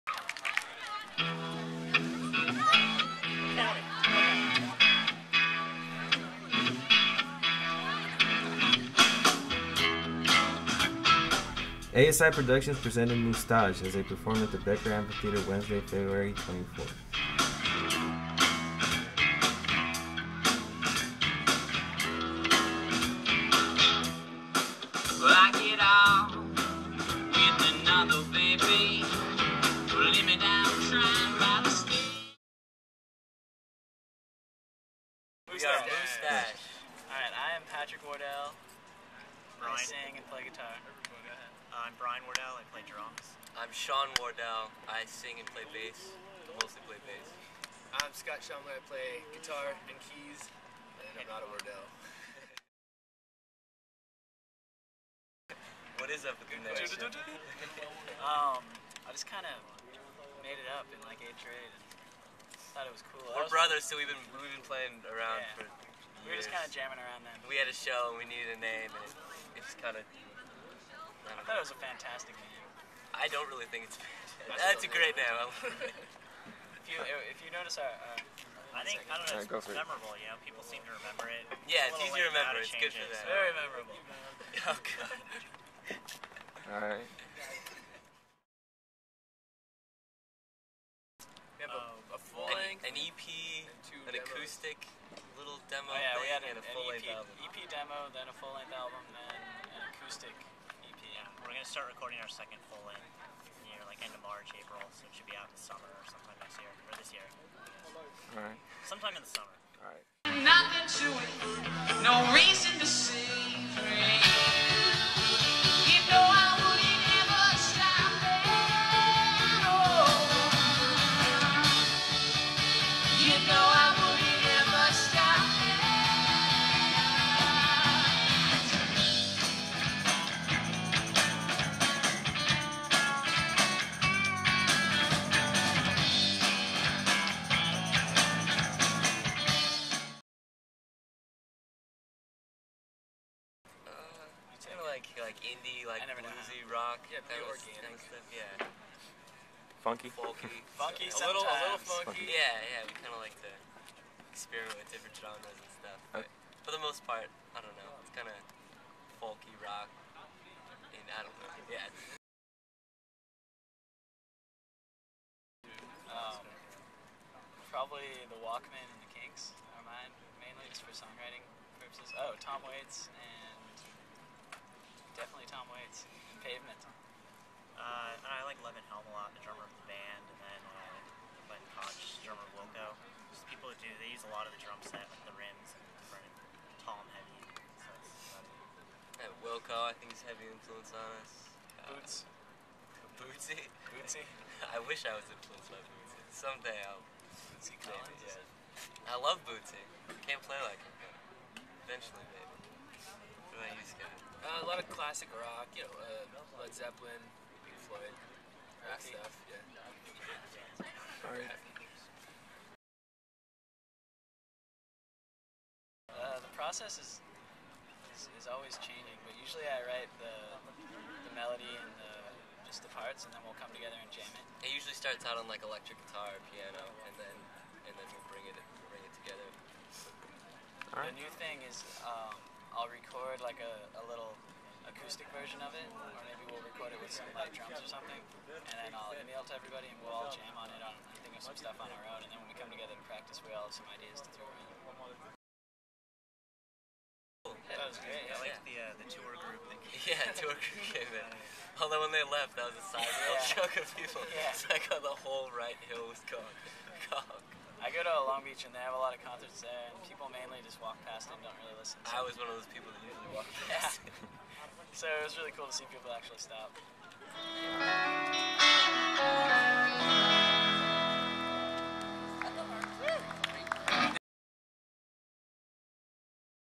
Moostache at Cal State Fullerton
Moostache comes to Cal State Fullerton to play at the Becker Amphitheatre on February 24 2010. Coverage by Oscar Romero